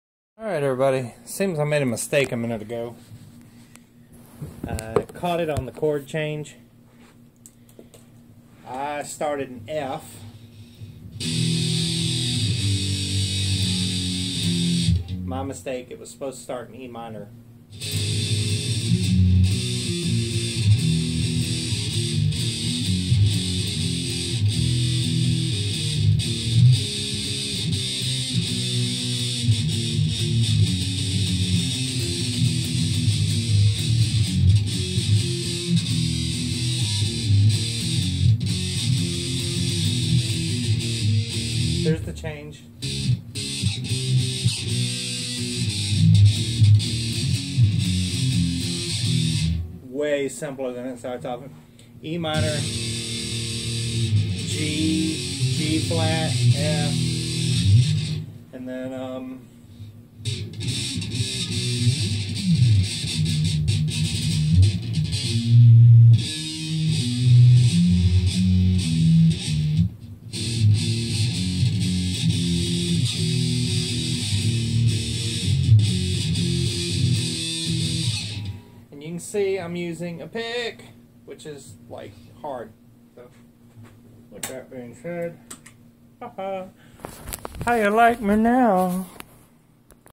Learning to Strum take 2
looked up the chord changes = I was off a ½ step too high = ease fix Start in Em' and not F'